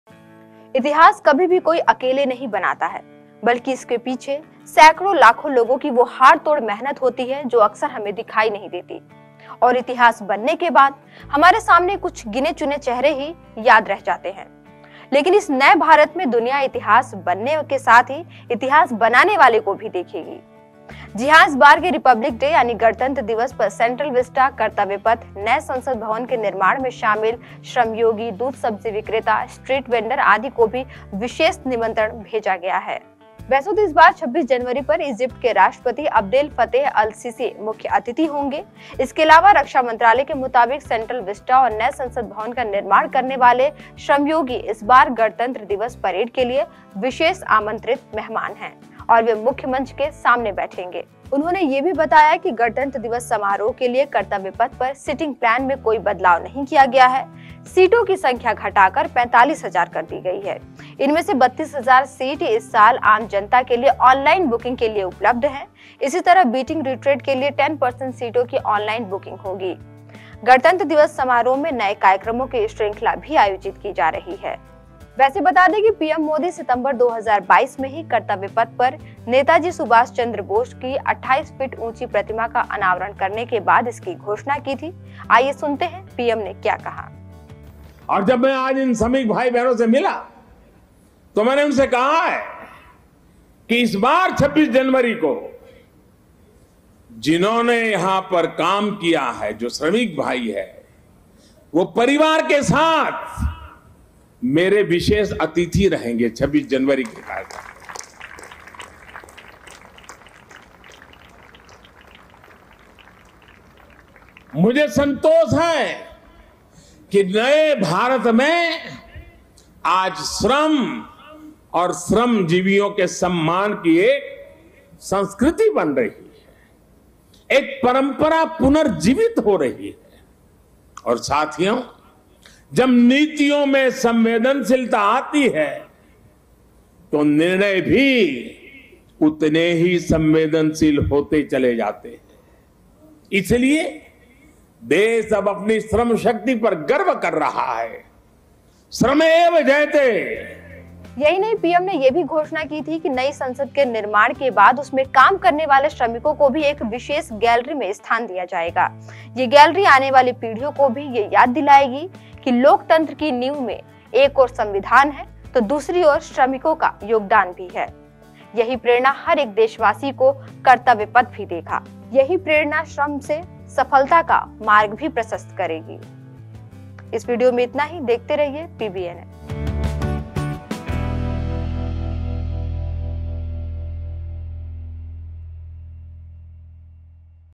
26 January: कर्तव्यपथ पर ‘श्रम योगी’ Republic Day के लिए विशेष मेहमान
इतिहास कभी भी कोई अकेले नहीं बनाता है बल्कि उसके पीछे सैकड़ों, लाखों लोगों की वो हाड़-तोड़ मेहनत होती है, जो अक्सर हमें दिखाई नहीं देती। और इतिहास बनने के बाद हमारे सामने सिर्फ कुछ-गिने चुने चेहरे ही याद रह जाते हैं। लेकिन इस नए भारत में दुनिया, इतिहास बनने वाले के साथ ही इतिहास बानाने वालों को भी देखेगी। जी हां इस बार के रिपब्लिक डे यानि गणतंत्र दिवस पर सेंट्रल विस्टा, कर्तव्य पथ, नए संसद भवन के निर्माण में शामिल श्रमयोगी, दूध- सब्जी विक्रेता, स्ट्रीट वेंडर आदि को भी विशेष निमंत्रण भेजा गया है।